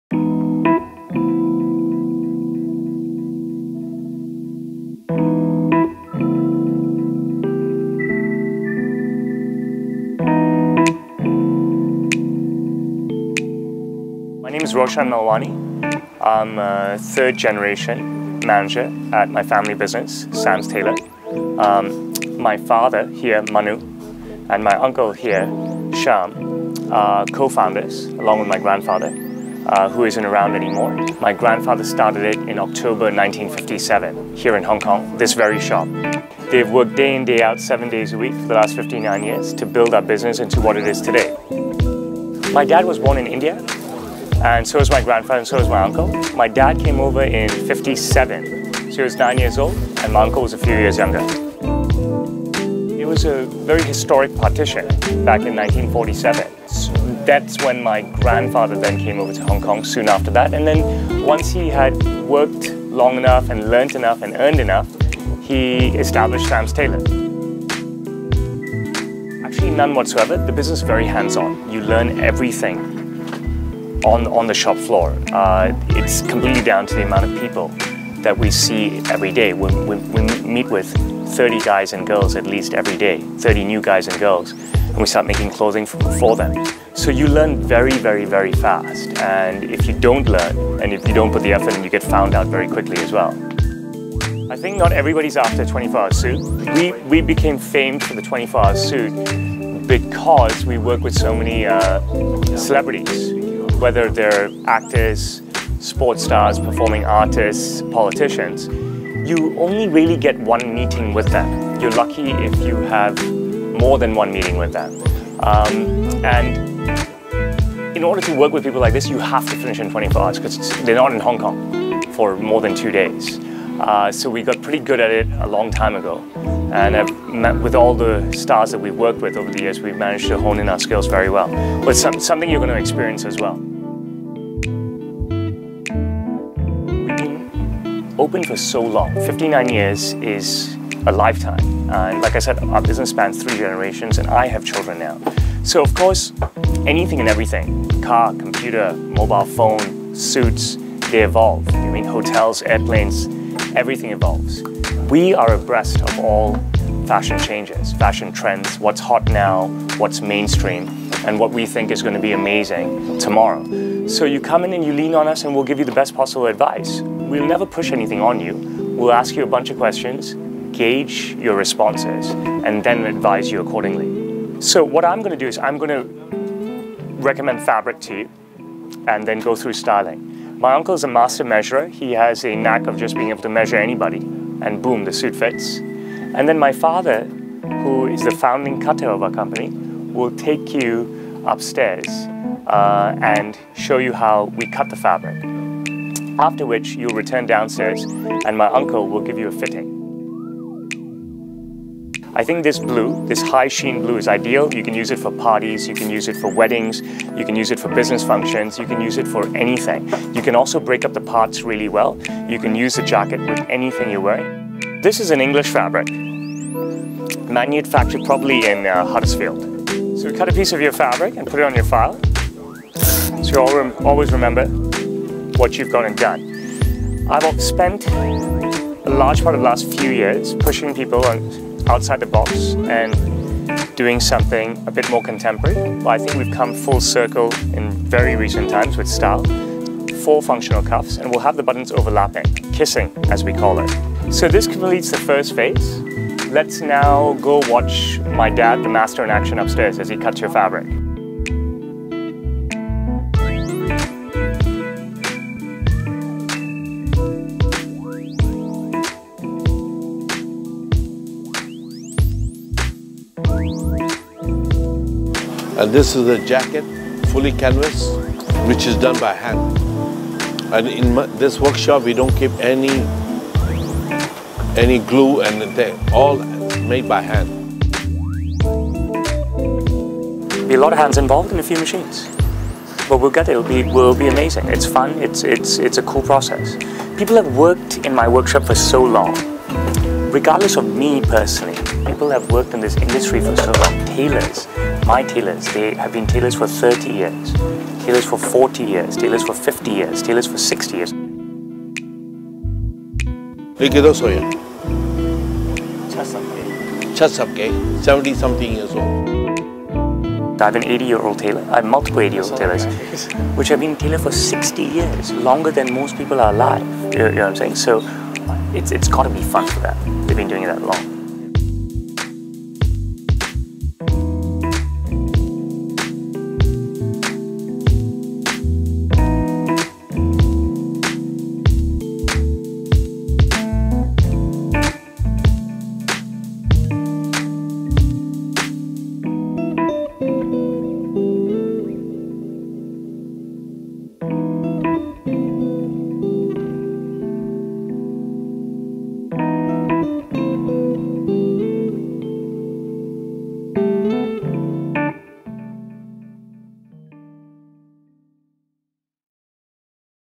Highsnobiety Visits The Story Behind Hong Kongs 24 Hour Suit
Highsnobiety Visited Sams to better understand what we do